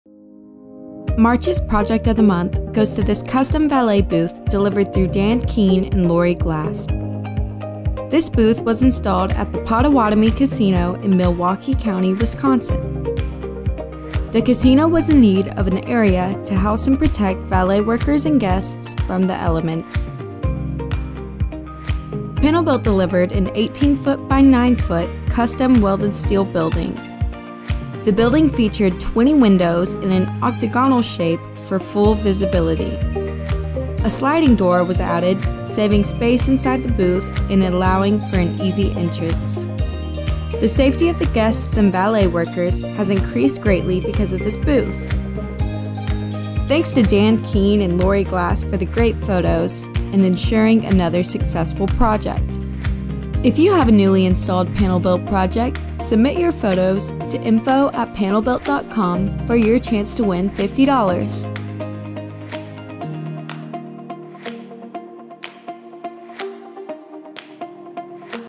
Fully Custom Valet Booth
March's project of the month goes to this custom valet booth delivered through Dan Keene and Lurie Glass.
This booth was installed at the Potawatomi Casino in Milwaukee County, Wisconsin.
The casino was in need of an area to house and protect valet workers and guests from the elements.
Panel Built delivered an 18' x 9' custom welded steel building. The building featured 20 windows and an octagonal shape for complete visibility. In addition, a sliding door was added, saving space inside the booth and allowing for an accessible entrance.
The overall safety of the guests and valet workers has significantly increased because of this booth.
Thanks to Dan Keene and Lurie Glass for the great photos and for ensuring another successful project.